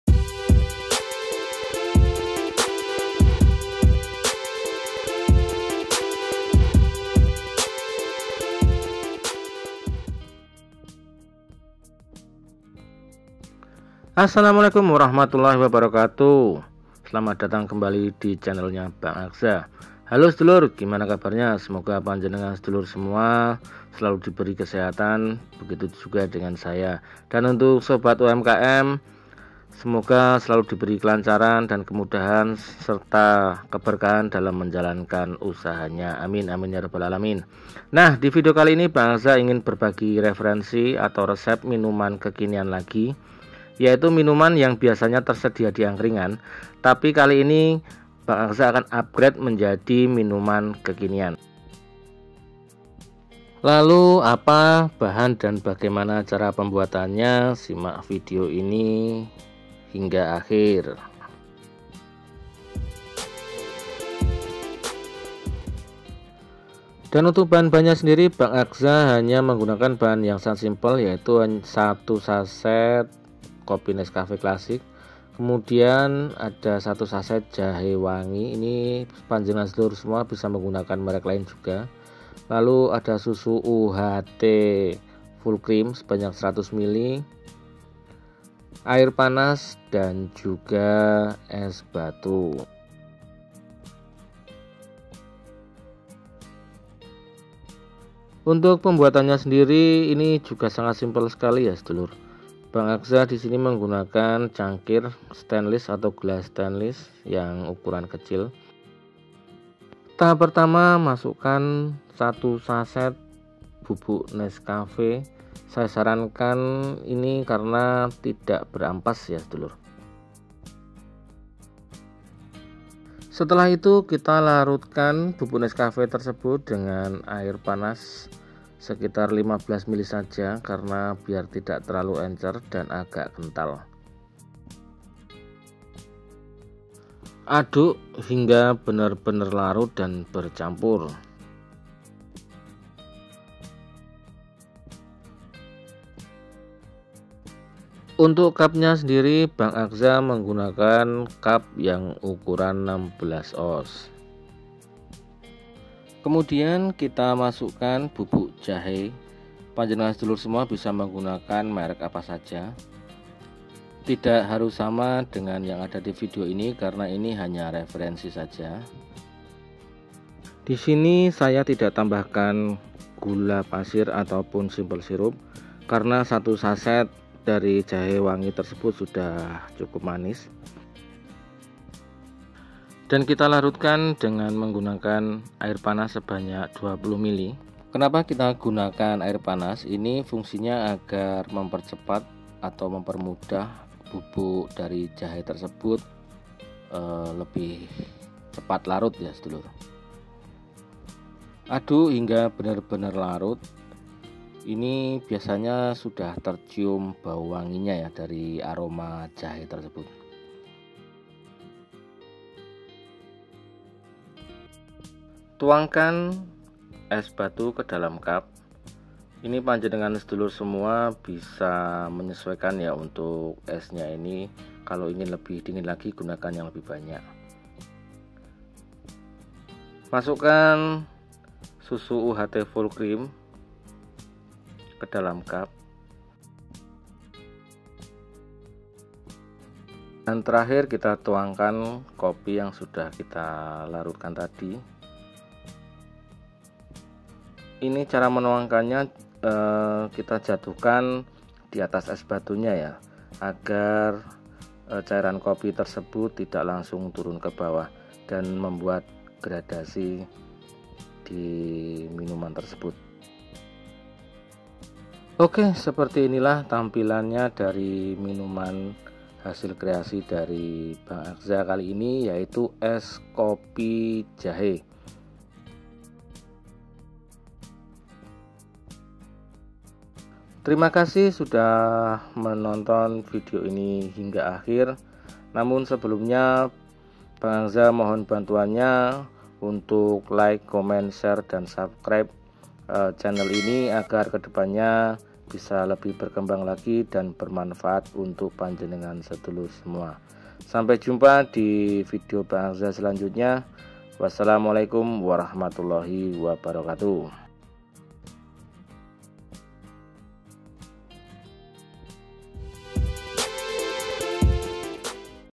MINUMAN ANGKRINGAN NAIK LEVEL‼️ES KOPI JAHE || IDE JUALAN MODAL KECIL MENJANJIKAN
Hallo sedulur dan sobat UMKM semoga kita semua di beri kesehatan dan kelancaran serta keberkahan dalam menjalankan usahanya... Aamiin 🤲 🤲 🤲

Di video kali ini @bangagza akan berbagi referensi atau resep minuman kekinian lagi yaitu ES KOPI JAHE atau istilahnya Minuman Angkringan di upgrade menjadi Minuman kekinian

Bahan :
1 Sachet Nescafe Clasic
1 Sachet intra Jahe wangi
Susu UHT Full Cream
Es Batu
Air panas secukupnya
Cup 16 Oz

Sekali lagi ini hanya referensi Saja Kalau cocok dan bermanfaat silahkan di kreasikan
Semoga bermanfaat untuk panjenengan sedulur semua
Terima kasih